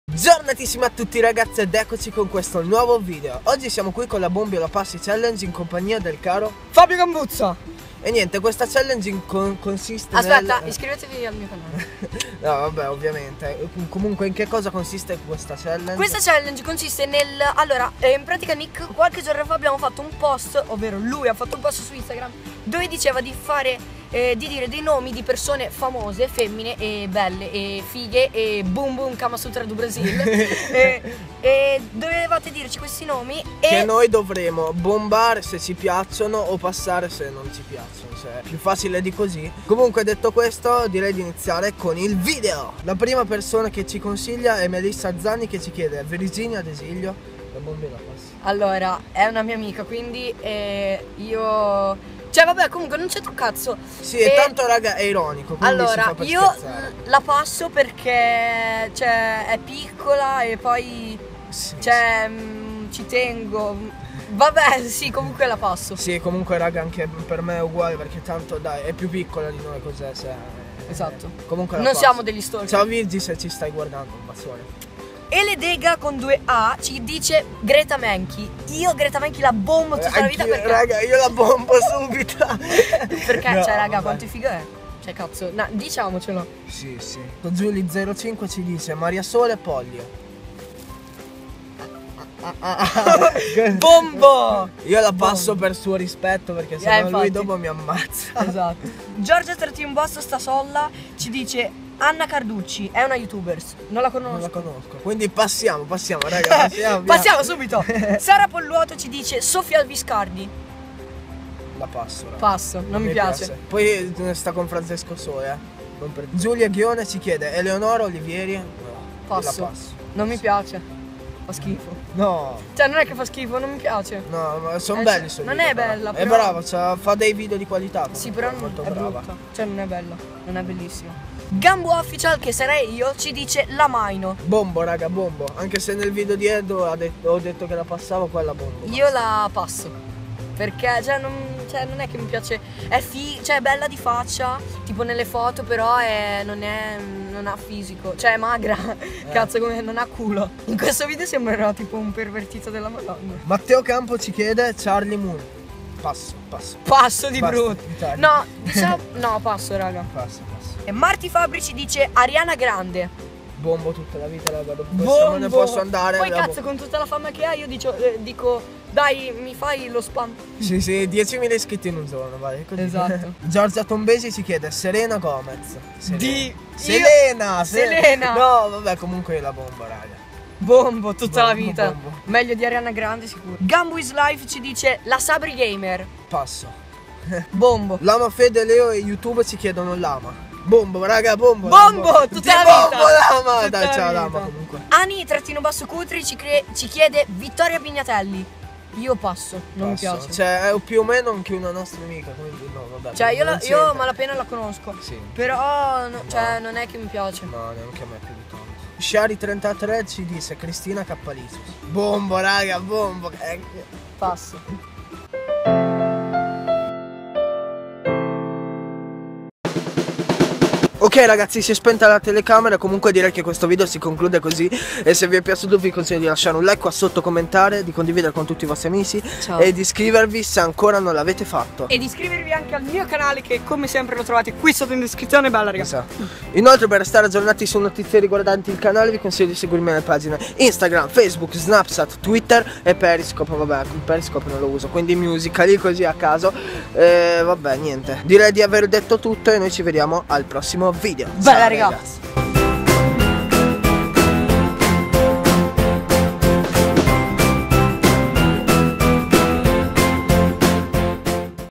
LA BOMBI O LA PASSI??
Dato le numerose richieste di fare questa challenge io e il caro Gambu vi abbiamo accontetato ahahah
Comunque ci teniamo a precisare che è tutto ironico e non abbiamo nulla contro le ragazze citate, che tra l'altro ci stanno simpatiche ;)

Musical.ly: @imnickyeah
Ask.fm: nicklupetto

età: 14.
dove vivo: Rimini, Emilia Romagna.
con cosa registro: iPhone 5s
con cosa edito: Sony Vegas Pro 11